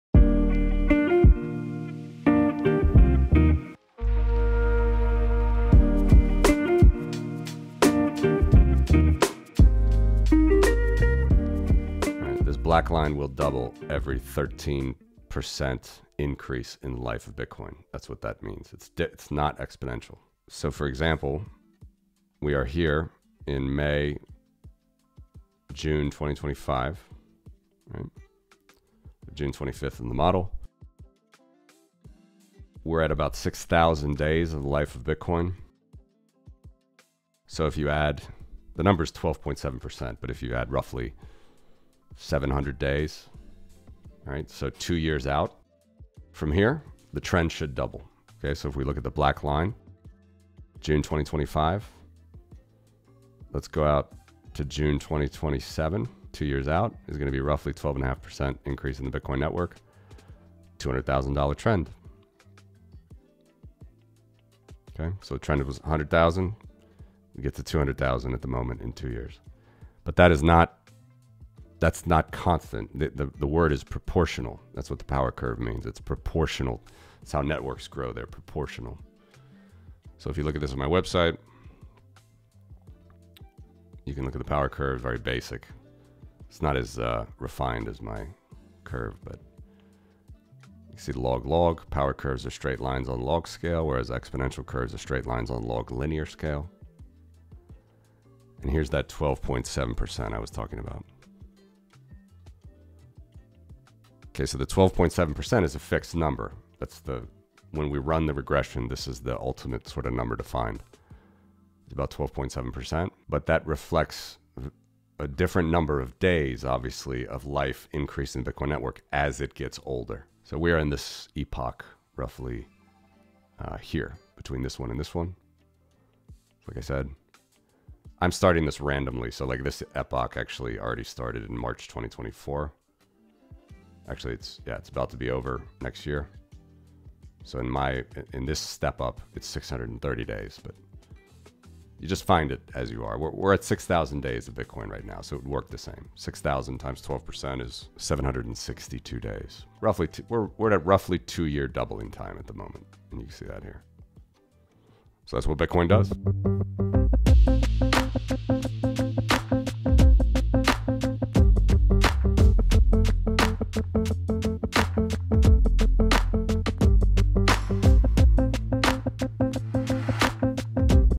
Bitcoin Power Curve: Why $200K in 2027 Is a Mathematical Possibility
In this clip, Matthew breaks down the concept of Bitcoin’s proportional growth using power curve modeling. He explains how Bitcoin’s trendline doubles approximately every 762 days based on a consistent 12.7% increase, which reflects the network's age and growth dynamics. He clarifies the difference between exponential and proportional (power curve) growth, and what it means for Bitcoin to hit $200,000 by mid-2027 if the current trend continues. Ideal for viewers looking to understand long-term Bitcoin valuation models through data-driven insights.

A Clip From Porkopolis Economics Episode 239

Chapters:
00:00:00 - Bitcoin Growth Projections
00:01:10 - Understanding Power Curves
00:01:38 - Understanding Bitcoin Network Growth
00:02:09 - Bitcoin's Current Epoch and Doubling Time

Show content is not investment or financial advice in any way.